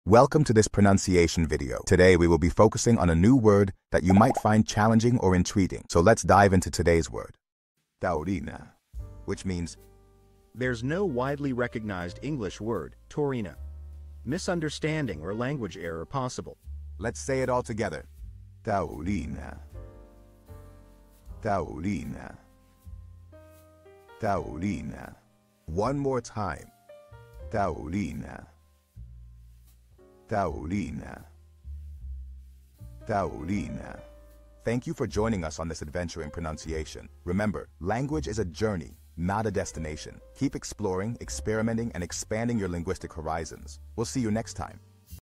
How to pronounce Taurina
Master the Pronunciation of 'Taurina Which means taurine' - which means : There's no Widely collected English Word "Taurina."Misunderstanding or Language Possible error. 📝 with @PronunciationSpanish 🎓🗣️ - [Your Guide to Spanish]

Learn to pronounce 'Taurina' perfectly in Spanish with our easy and engaging tutorial by @PronunciationSpanish! 📖📽️ In this comprehensive video, we provide you with native speaker examples 🗣️👂 and phonetic breakdowns 🎵 to ensure you get the pronunciation just right.

How to say Taurine in Spanish